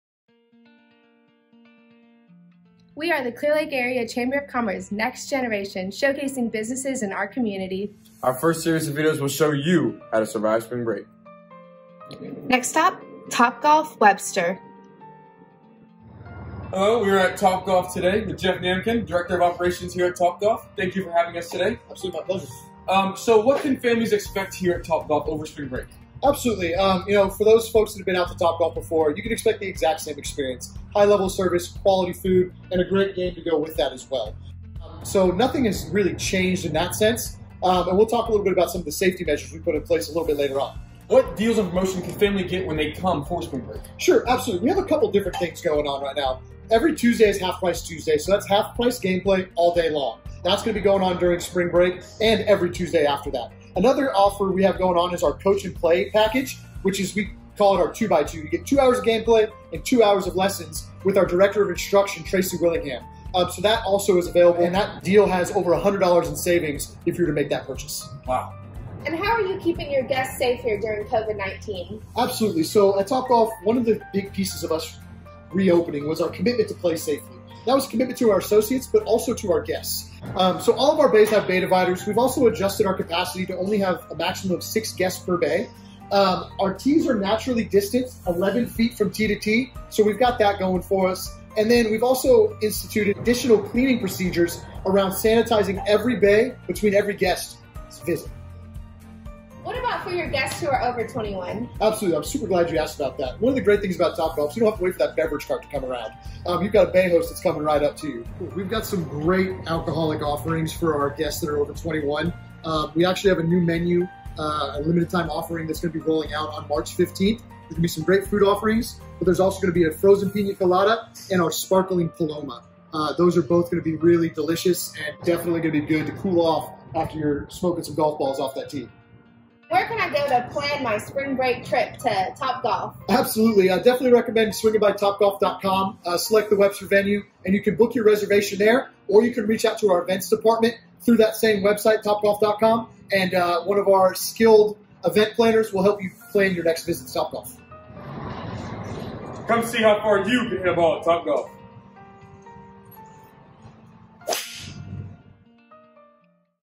Spring Break Survival | Topgolf
Families can expect the same great experience of high-level service, quality food and a great game at Topgolf in Webster this Spring Break. Every Tuesday is half price Tuesday, which means there is half price game play all day long, all year long! They also currently have their Coach & Play package going on that goes by 2x2, meaning you get two hours of lessons followed by two hours of game play with their director of instruction. For their guests who are over 21, there are rolling out a new limited time cocktail menu on March 15th. This menu includes a frozen pina colada, sparkling paloma and some grapefruit offerings to quench that mid-game thirst. In light of the Covid-19 pandemic, Topgolf’s main goal is their commitment to play safely. They have placed bay dividers and adjusted their capacity to 6 guests per bay. With their tee’s naturally 11 feet apart, there is plenty of room for swinging golf clubs and having some Spring Break fun. For more information about booking a reservation, swing by topgolf.com and pick the Webster location or reach out to their events department at and one of their lovely event planners will help you plan your next visit.